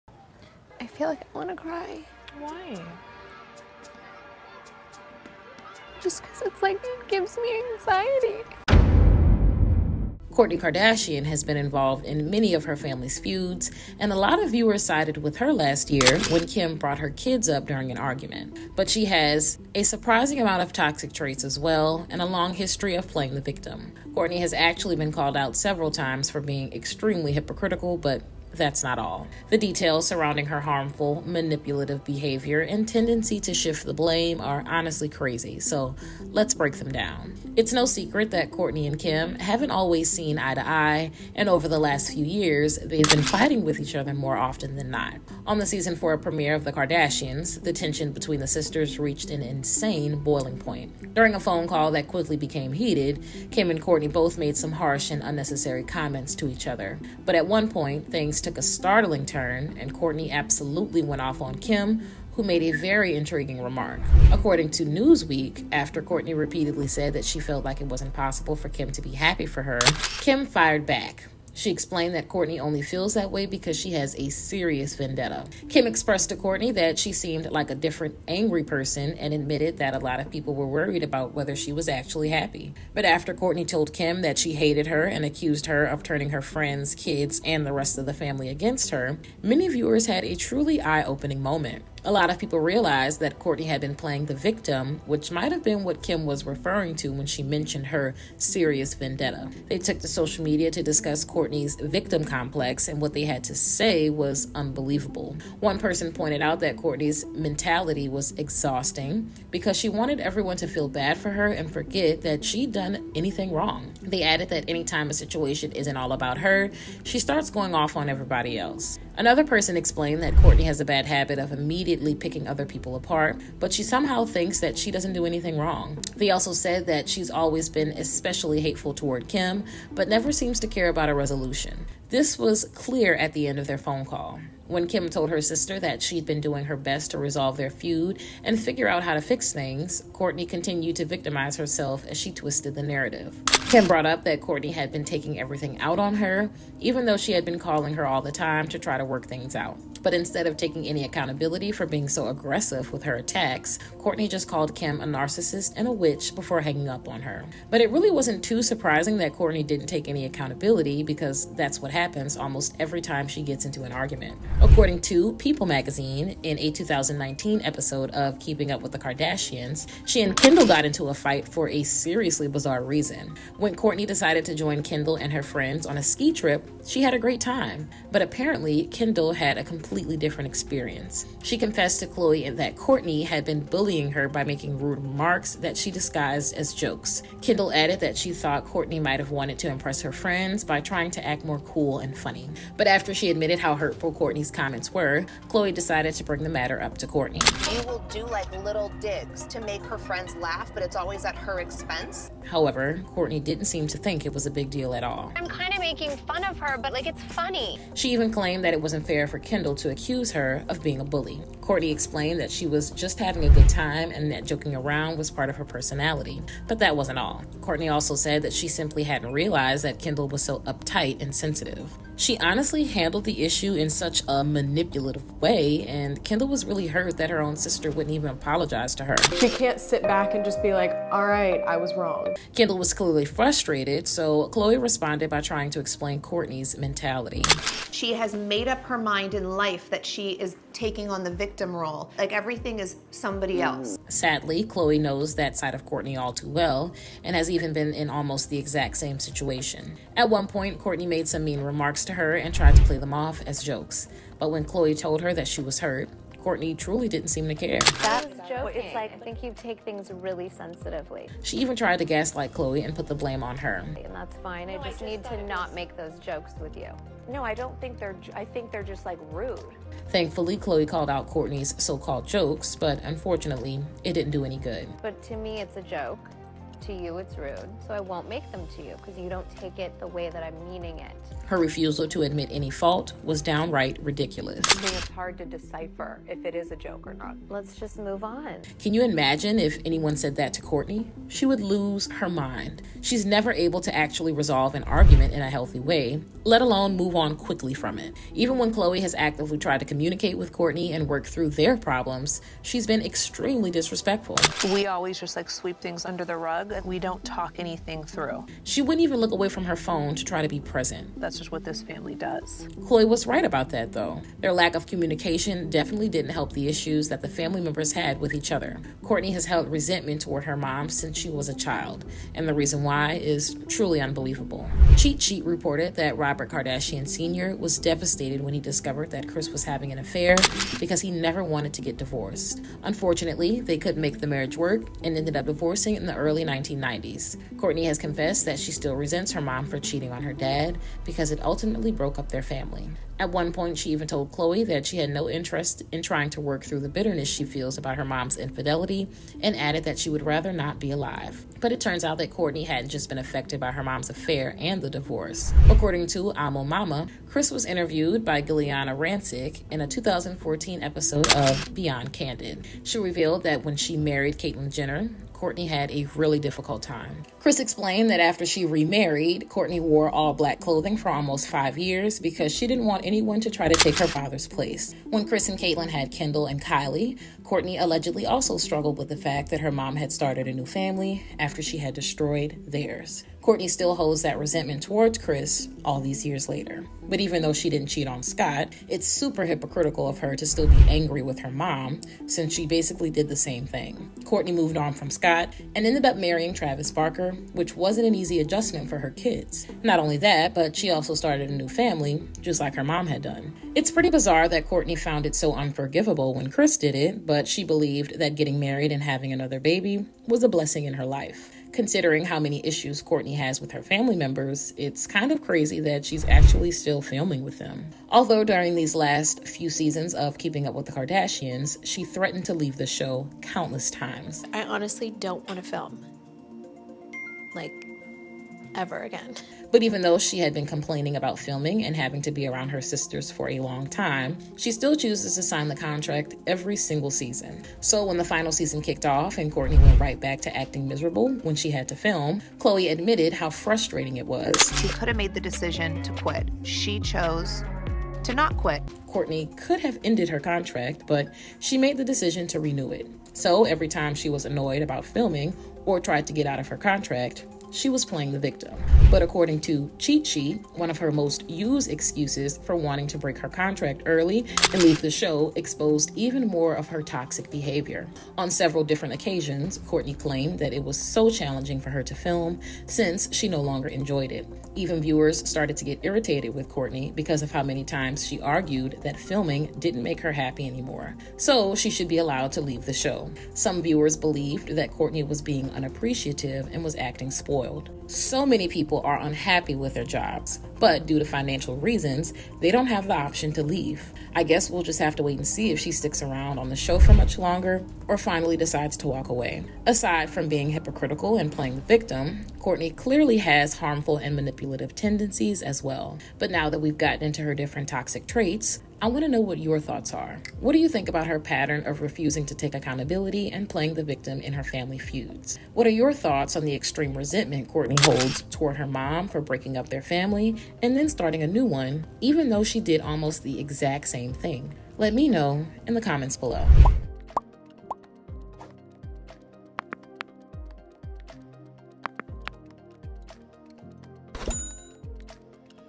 Exposing the Truth About Kourtney
Unveiling Kourtney's Toxic Traits

We'll explore the ongoing family drama between Kourtney Kardashian and her sister, Kim. While Kourtney garnered sympathy from fans during a heated argument last year involving their kids, she has also developed a reputation for playing the victim. In one particularly explosive phone call, Kourtney accused Kim of turning their friends, kids, and family against her, causing viewers to take notice of her victim mentality. Fans quickly expressed their frustrations online, pointing out Kourtney’s tendency to shift blame and make everything about herself.